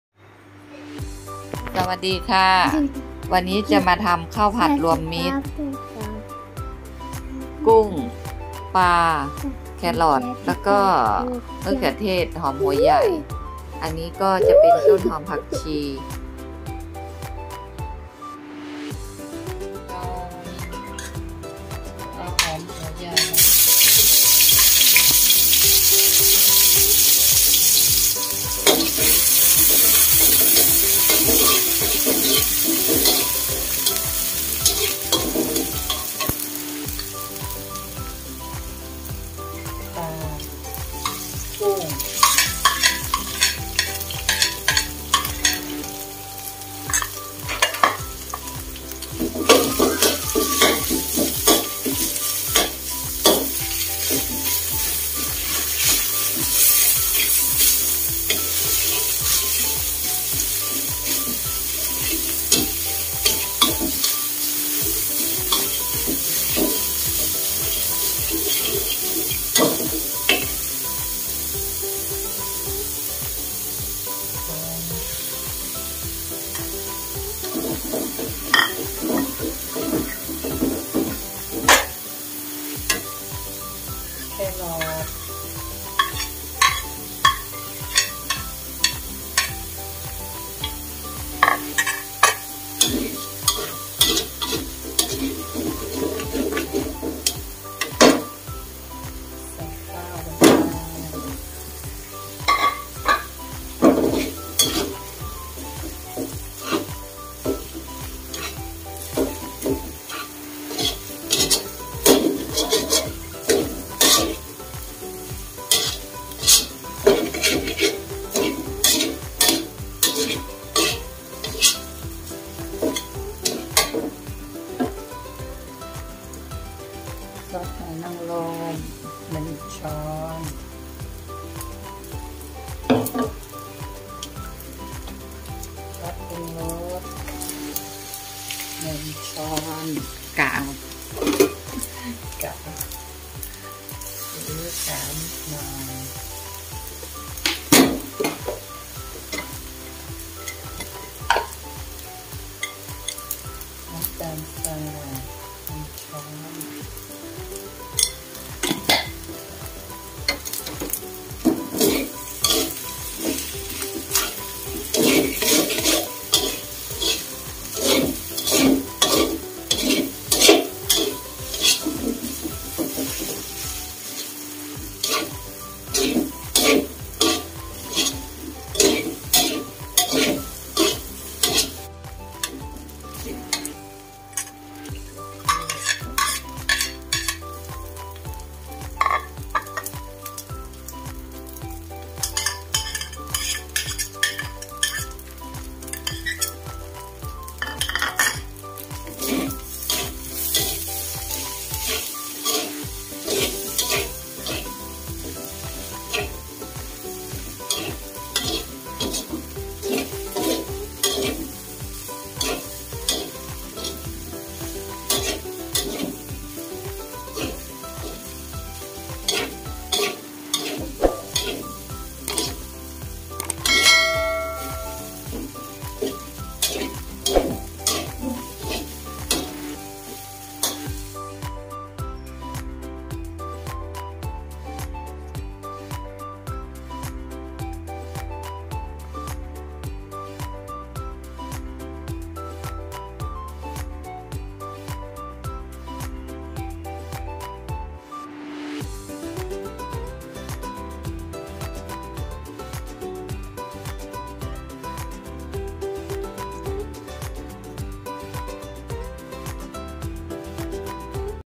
เมนูง่ายๆ ข้าวผัดรวมมิตร กินได้อร่อยด้วย ข้าวผัดน้องคุณ
ข้าวผัดให้น้องคุณ เด็กๆกินง่าย ทำง่ายๆก็อร่อย
ปลาแซลมอน กุ้งสดสับ
หอมหัวใหญ่สับ
มะเขือเทศหั่นฝอย
แครอทหั่นฝอย
ต้นหอมผักชีหั่นฝอย
ซอสหอยนางรม ผงปรุงรส
นํ้าตาล ซีอิ๊วขาว ซอสฝาเขียว